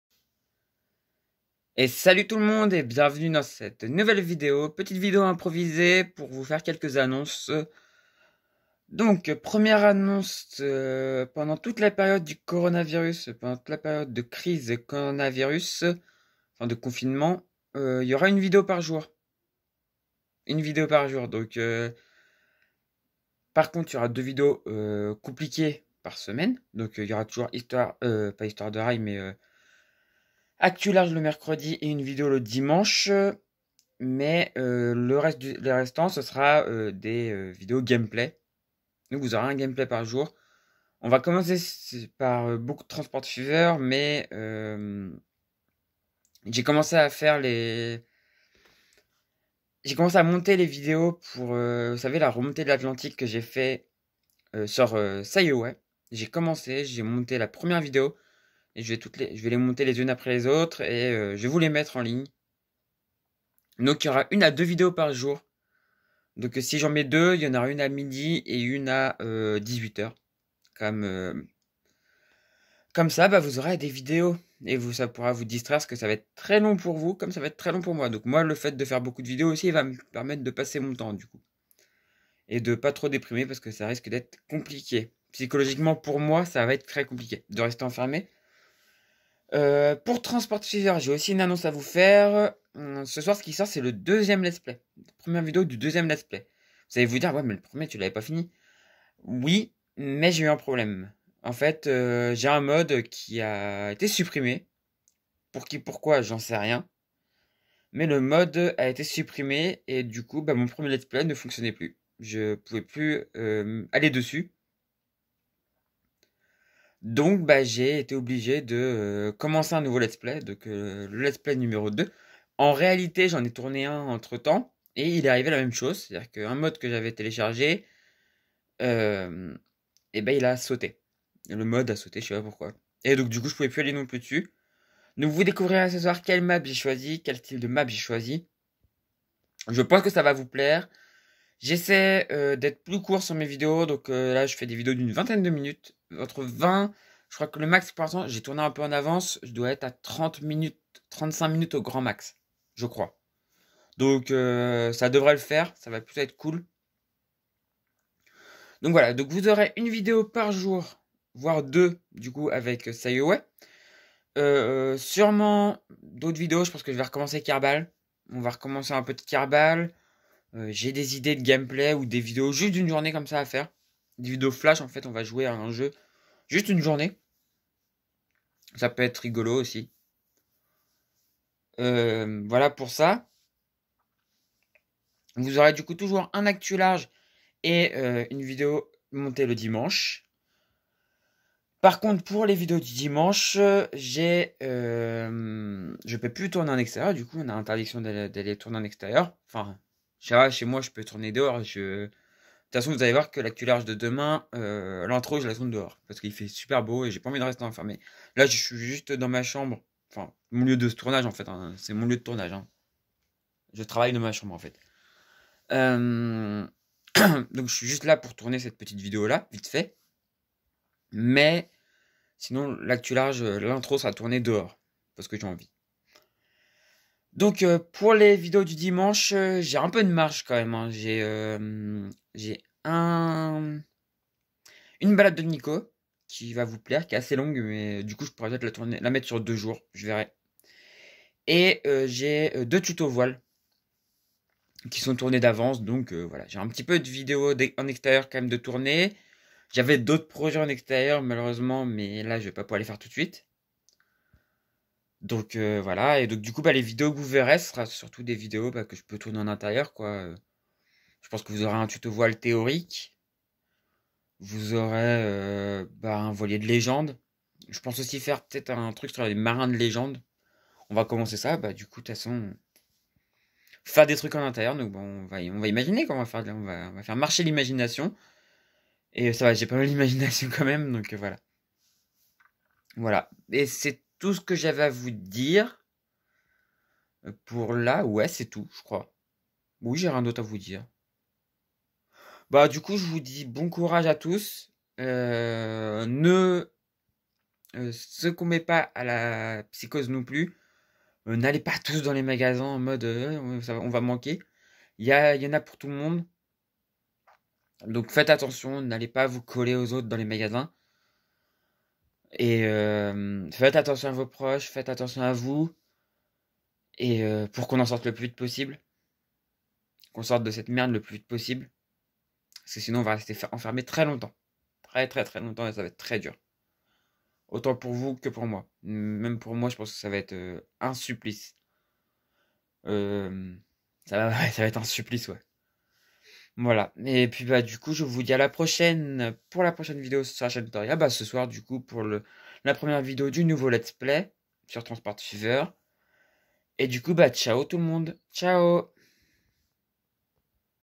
Information sur la suite de la chaîne
Je vous donne des informations sur ce qui va ce passer sur la chaîne pendant le confinement